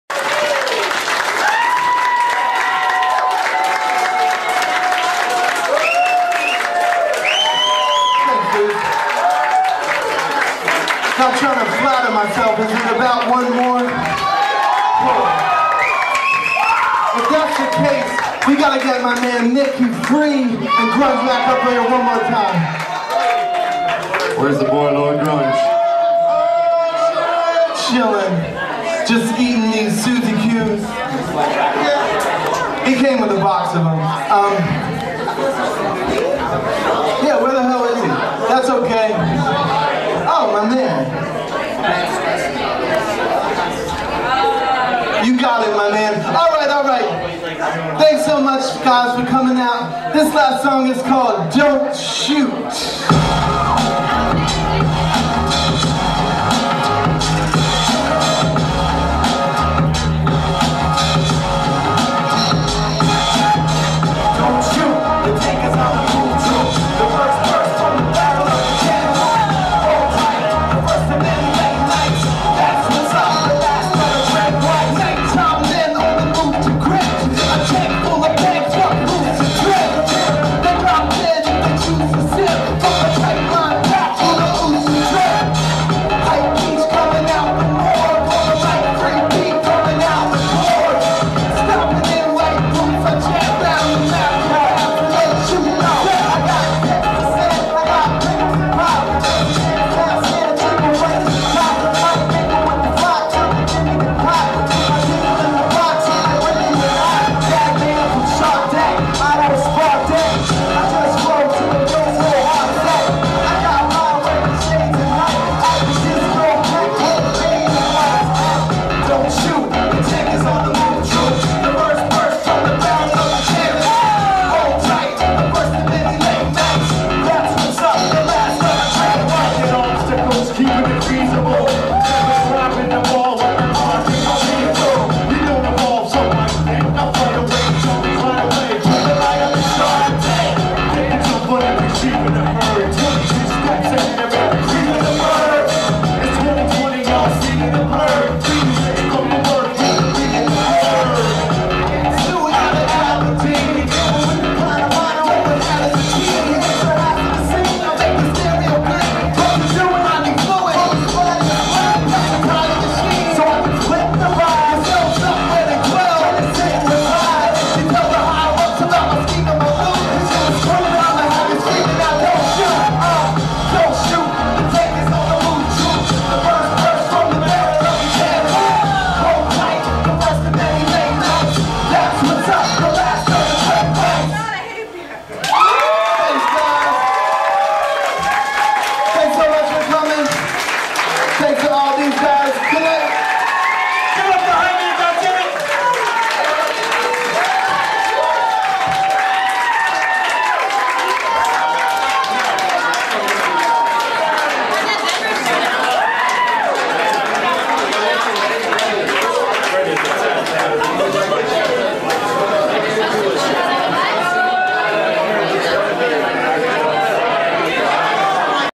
Height with Friends @ Hexagon -- 5.28.10 -- Bed of Seeds tour -- part TWO
Featuring -- Height with:
Lord Grunge
Mickey Free

"live from Baltimore, Maryland, USA, Eaarth, Solar System, Milky Way"
video by DNA

Thanks to Dan and Friends Records and the nice folks at Hexagon who let me in to tape despite a sold-out show in a swealtering space!